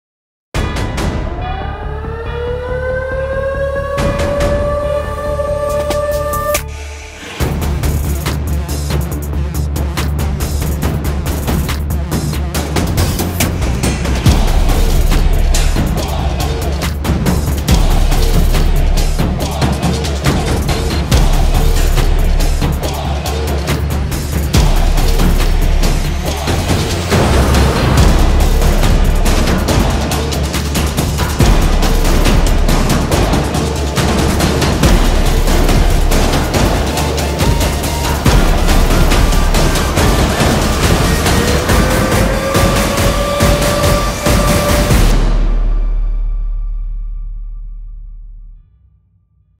2016-17 Dayton Flyers Men's Basketball Hype Video
The 2016-17 Dayton Flyers preseason hype video. True Team Basketball.